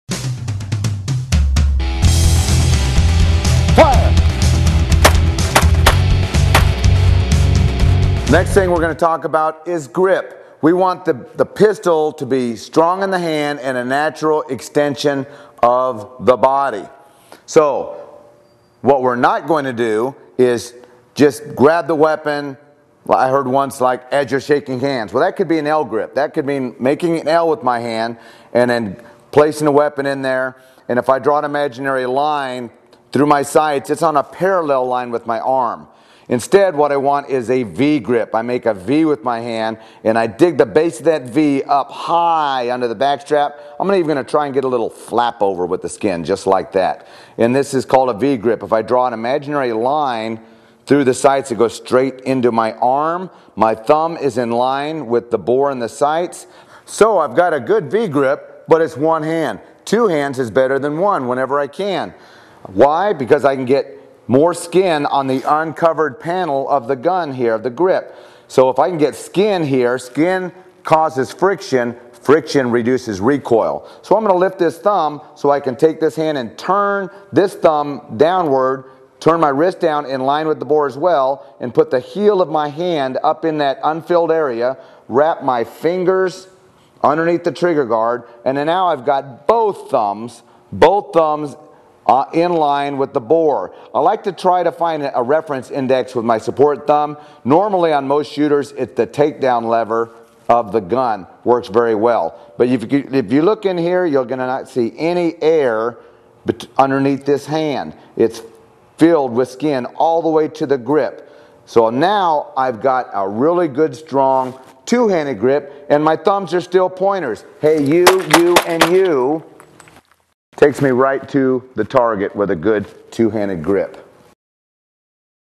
USA AMMO Handgun: The Grip
Brian Hoffner explains the best way to hold your weapon to reduce recoil and keep the gun in the fight.